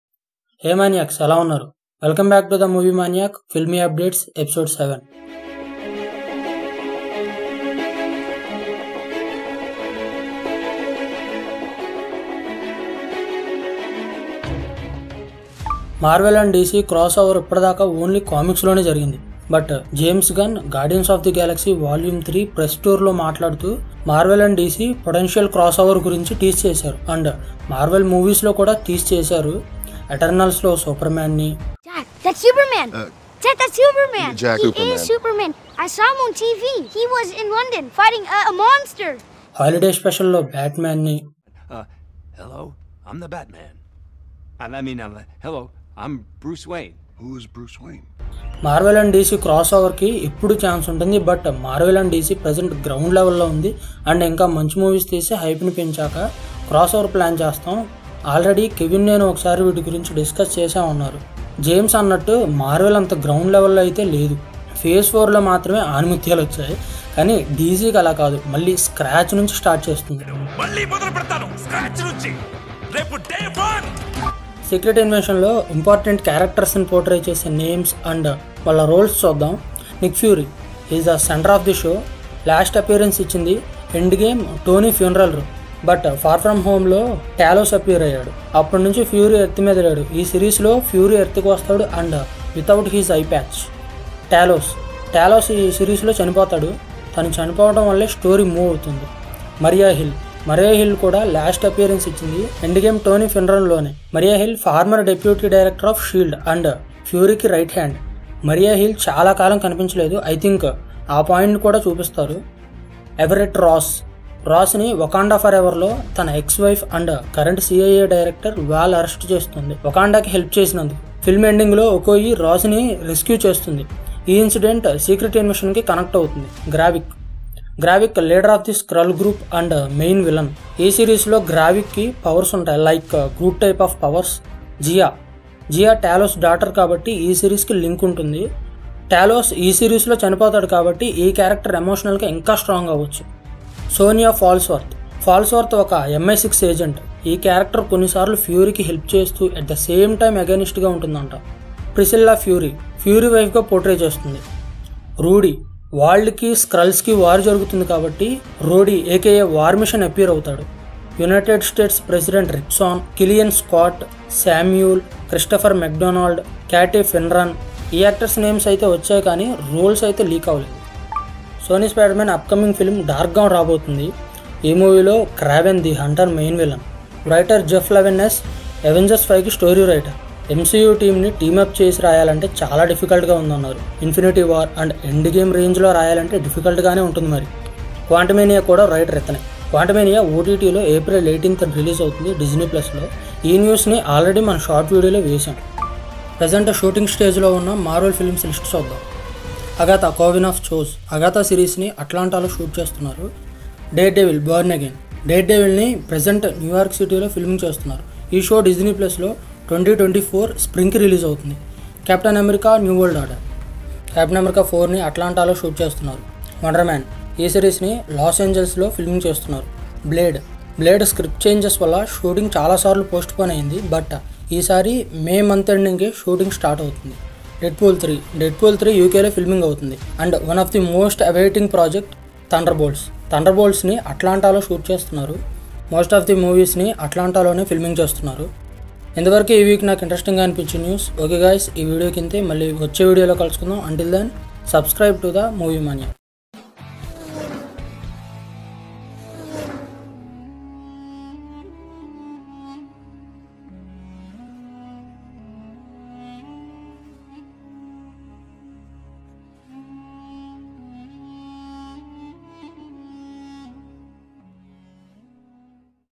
Marvel-DC Crossover Convergence || Filmy News ep-7 || Movie Maniac
In today's video, we have some exciting news to share with you.

First up, we have some buzz around a possible Marvel-DC crossover event! While it's still just a rumor, fans are abuzz with excitement over the possibility of seeing some of their favorite heroes from both universes teaming up. Could we finally see the likes of Superman and Captain America fighting side by side? Only time will tell!

Next, we have some details about the upcoming Disney+ series, Secret Invasion. The show's main characters have been revealed, with Samuel L. Jackson returning as Nick Fury and Ben Mendelsohn reprising his role as Talos. We can't wait to see what kind of shenanigans these two get up to as they investigate a potential Skrull invasion.

Moving on to Sony's upcoming Spider-Man film, we have some exciting casting news! The film will reportedly feature Charlie Cox as Daredevil, suggesting that the movie will explore the multiverse and potentially connect to the wider Marvel Cinematic Universe.

Lastly, we have some updates on the current shooting stage movies of the MCU. From Black Panther: Wakanda Forever, Thor: Love and Thunder, Doctor Strange in the Multiverse of Madness to Ant-Man and the Wasp: Quantumania, we have some exciting details on the storylines, cast, and filming locations.

We hope you enjoyed this quick rundown of the latest Marvel and DC news.